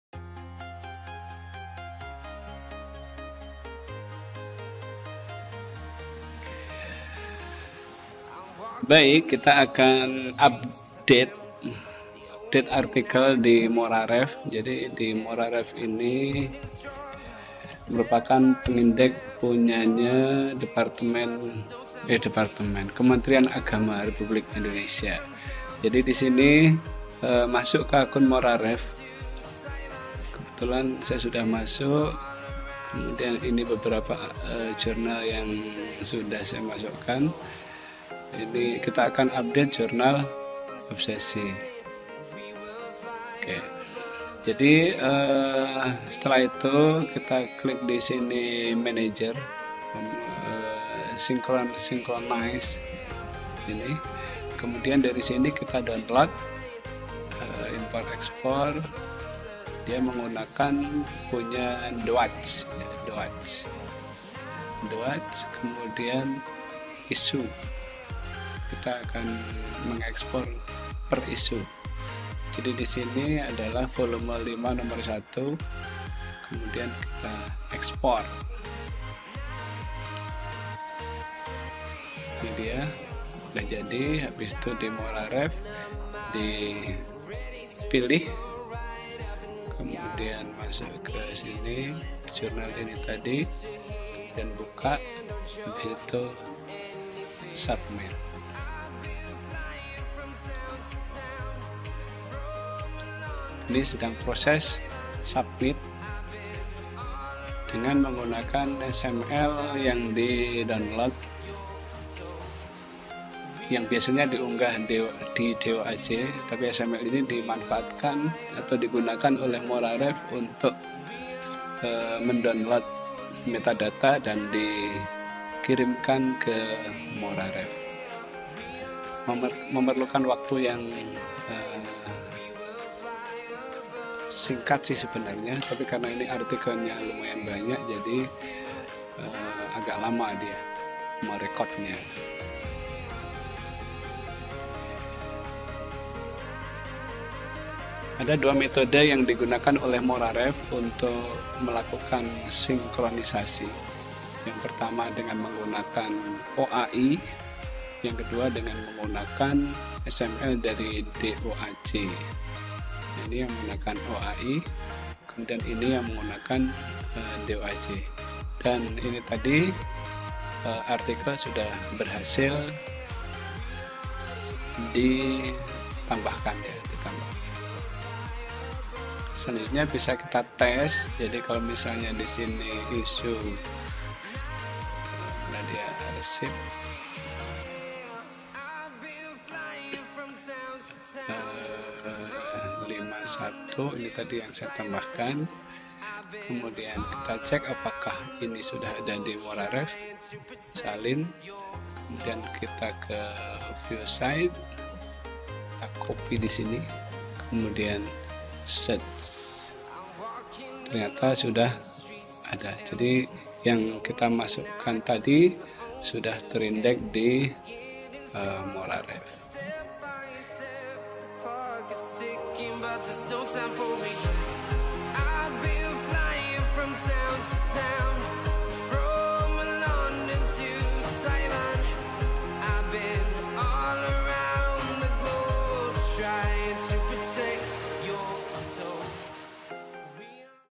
Submit Artikel ke Moraref
Moraref merupakan salah satu pengindeks jurnal milik Kementerian Agama RI, dalam video ini adalah cara submit artikel jurnal ke moraref dengan menggunakan format xml DOAJ

Langkah-langkah Indeks artikel jurnal di Moraref adalah
1. Register ke Moraref
2. Tunggu konfirmasi akun di email anda
3. Buka akun Moraref
4. Tambahkan jurnal yang akan diindeks
5. Lengkapi data yang diperlukan
6. Submut artikel dengan dua metode yaitu OAI dan XML DOAJ
7. Selesai